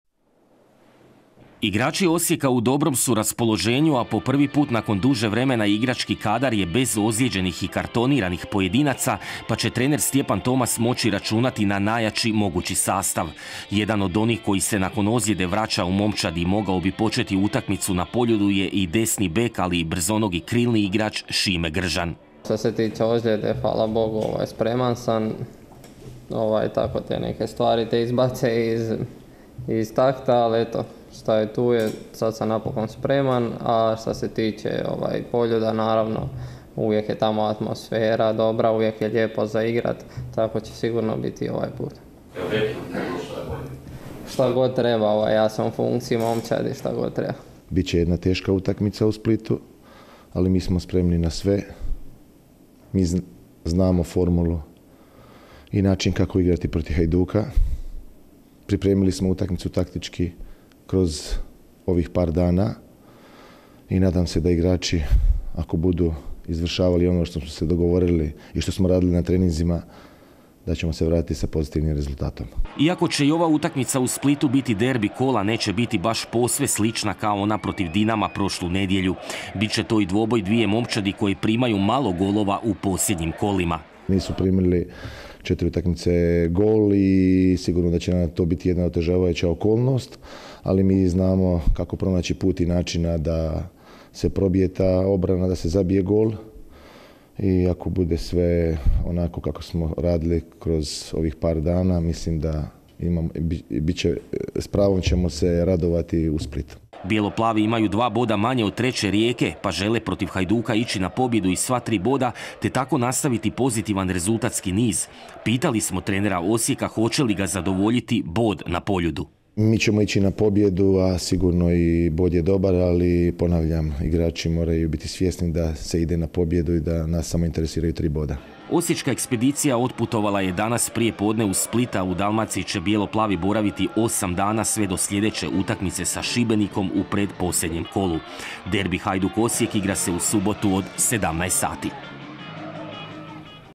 Tomas najavio igru na pobjedu protiv Hajduka na Poljudu
Nogometaši Osijeka u subotu će u derbiju 34. kola Super sport HNL-a gostovati na Poljudu kod Hajduka. Osječani žele u preostala 3 kola prvenstva osvojiti maksimalan broj bodova i osigurati treće mjesto, iako i četvrta pozicija vodi u europska natjecanja.